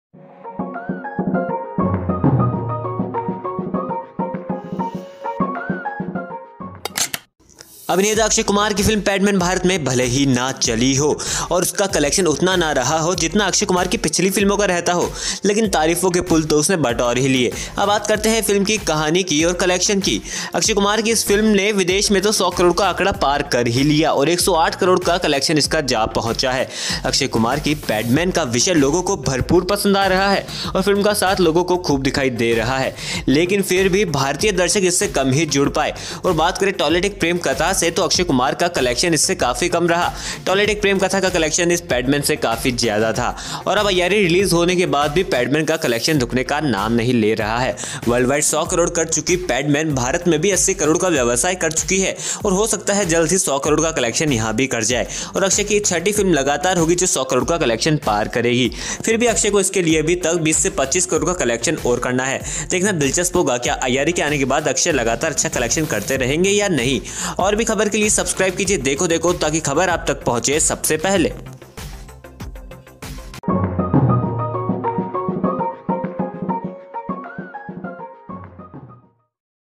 Padman Worldwide पर Padman Akshay Kumar ने Box Office पर बनाई Century,Padman Collection
Padman Worldwide 108 Crores पर Padman Akshay Kumar ने Box Office पर बनाई Century,Padman Collection